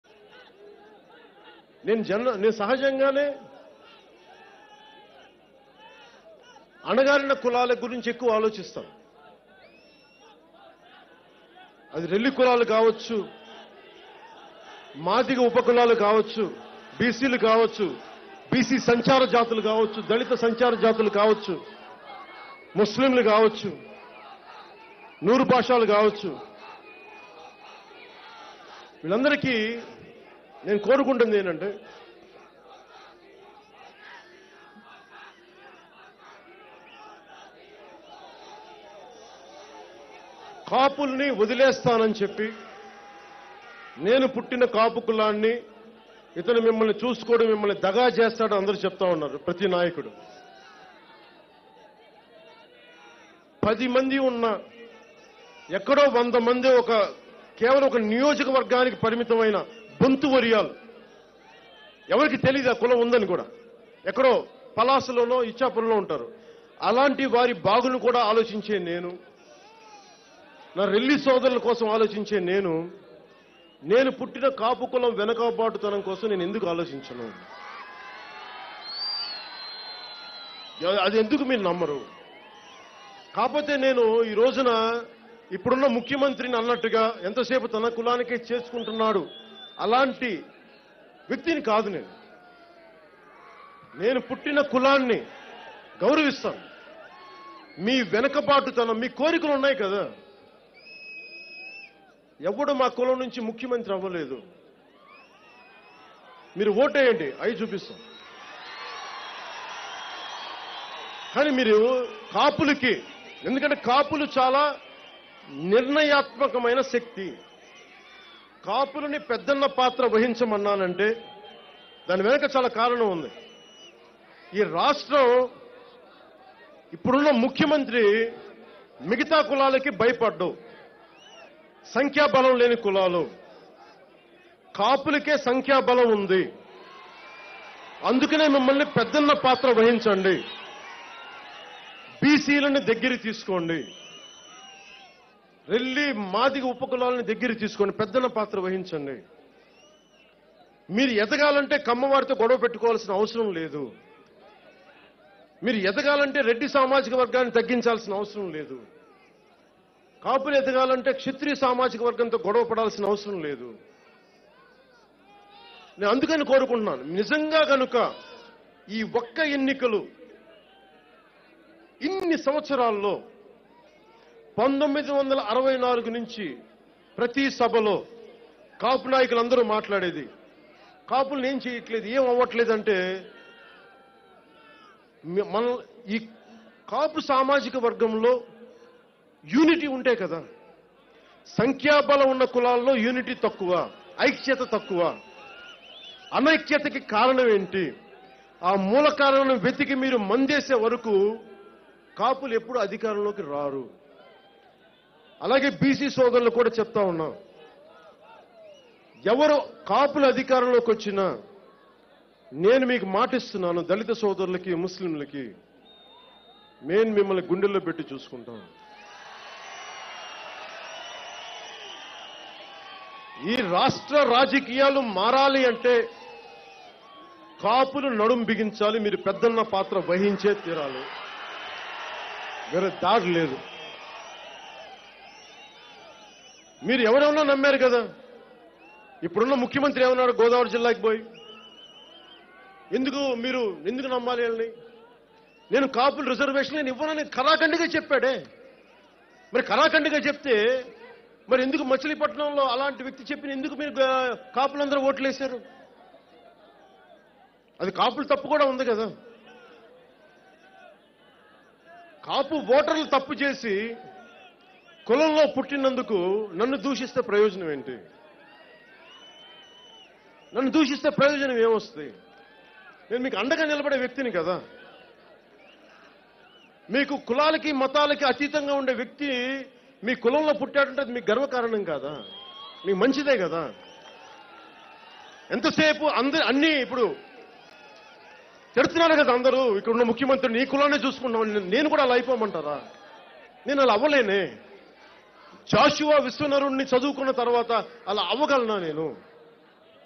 కాపులు తలుచుకుంటే నేను సీఎం అయ్యి చూపిస్తా | PawanKalyan Mind Blowing Words On Kapu Leaders
LIVE🔴 బందరు లో దుమ్ములేపుతున్న జనసేన ఆవిర్భావ సభ ఏర్పాట్లు | Janasena Formation Day Arrangements